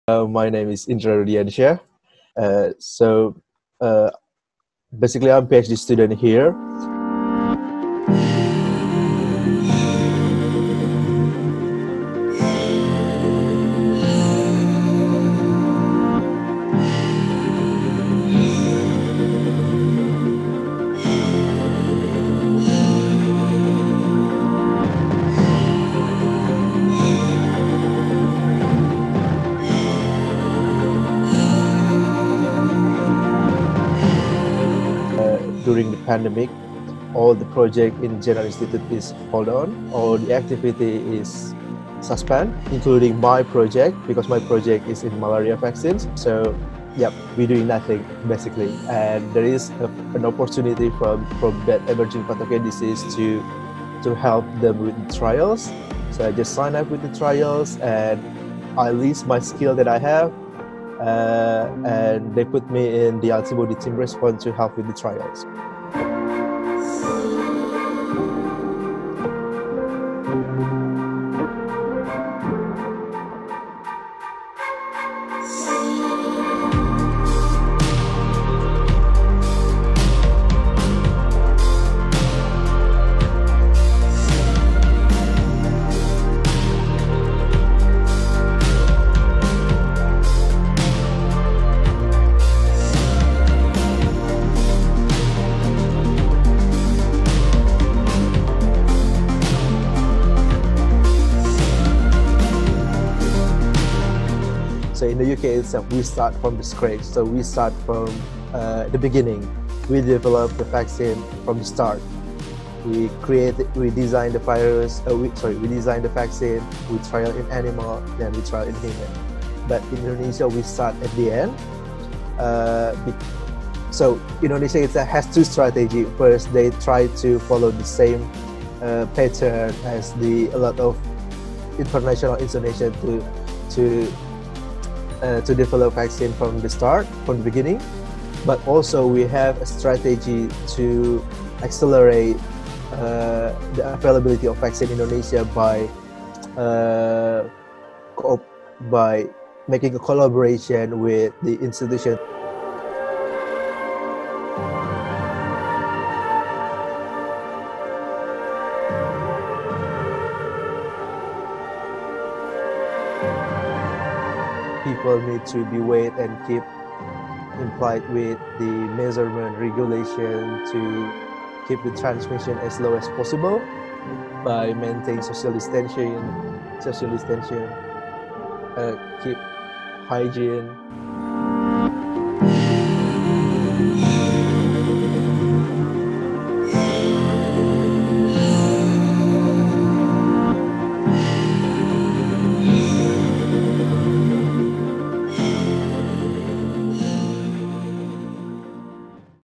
Meet the Indonesian student on Oxford's COVID-19 vaccine team
Indonesian PhD student Indra Rudiansyah is one of 297 researchers on the University of Oxford’s COVID-19 vaccine team. Indra completed his bachelor’s and master’s degree at Bandung Institute of Technology (ITB) before going to Oxford in 2018  to pursue a doctorate in clinical medicine, with a focus on the malaria vaccine. After the  pandemic halted Indra’s research project, he applied to join the vaccine development team, where he is responsible for collecting antibody response data.